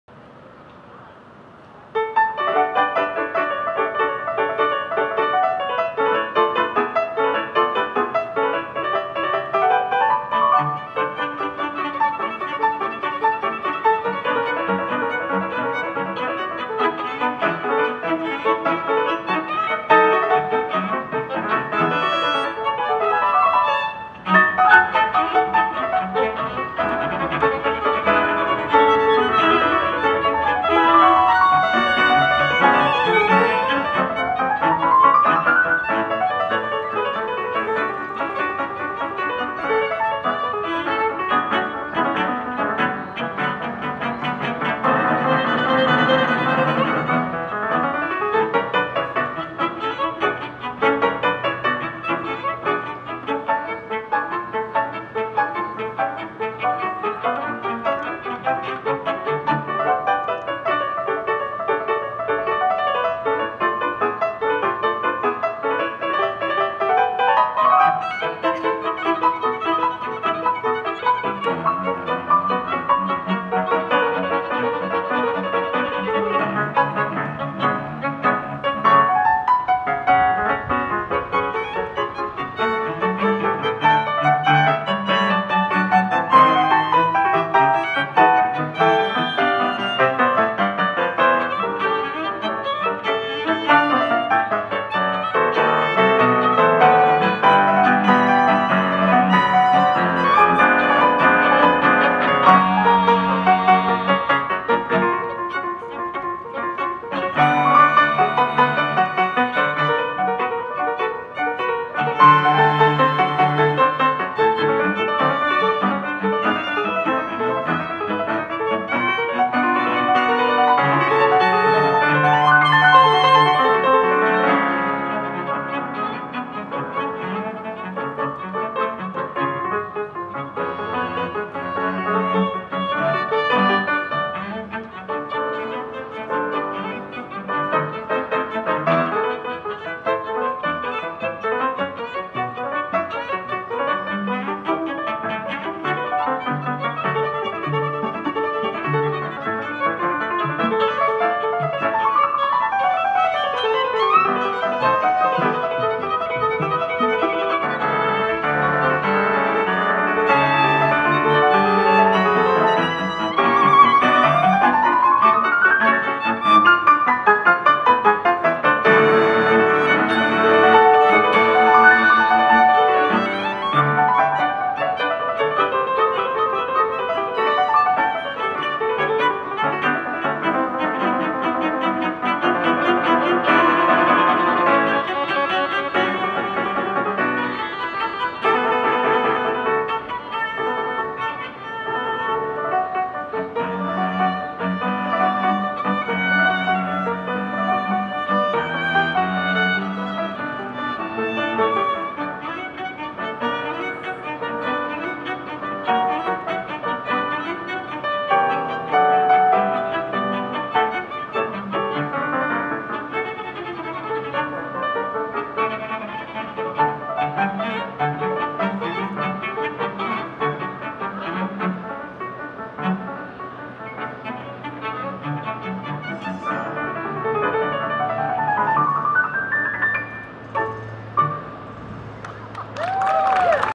Classical Concert
This is a concert performed at the Pioneer Courthouse Square in Portland, Oregon. September 27, 2020